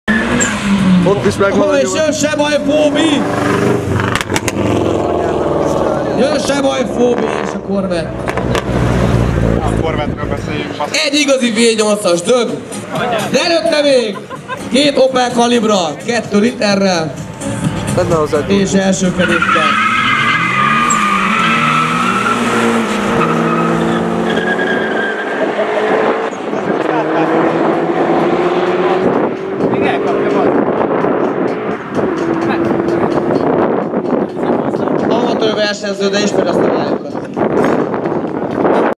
Opel Calibra Turbo vs Opel Calibra Turbo C20let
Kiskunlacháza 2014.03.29
Calibra Turbo Ph3 FWD vs Calibra Turbo Ph3 FWD
Ph3 (green): 14.5
Ph3 etanol ( blue): 14.6